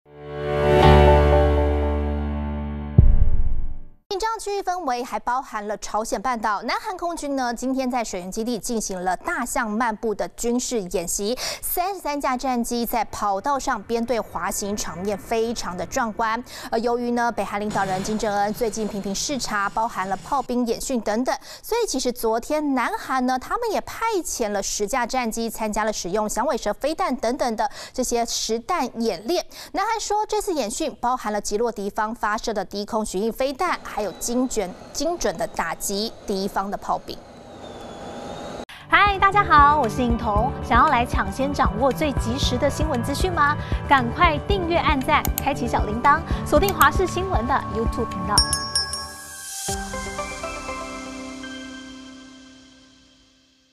南韓空軍33架戰機「大象漫步」演習 場面壯觀！｜華視新聞 20240308
南韓空軍今(8)日在水原基地，進行了「大象漫步」軍事演習。